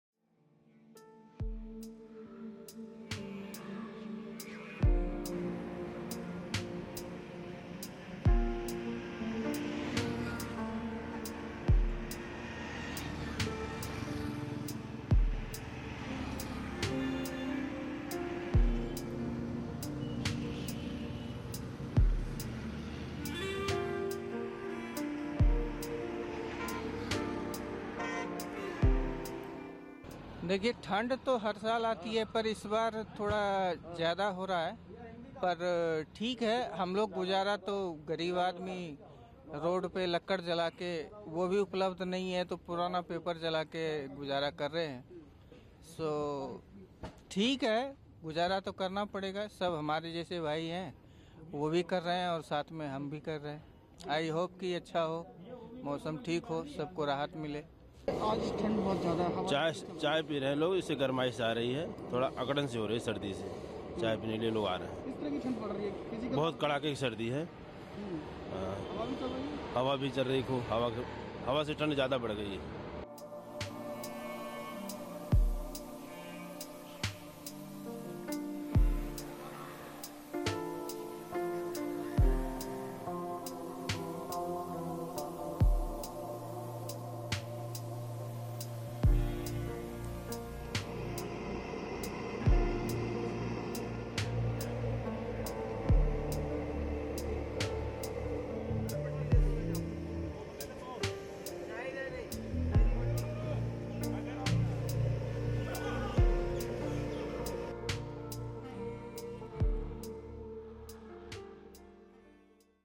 Visibility drops as fog shrouds in Delhi
New Delhi, Dec 17 (ANI): Fog continued to envelop in the national capital on December 17, leading to low visibility. As per India Meteorological Department (IMD), cold wave conditions are very likely in some pockets over Punjab, Haryana, Chandigarh, Rajasthan, Delhi and West Uttar Pradesh during next 2 days.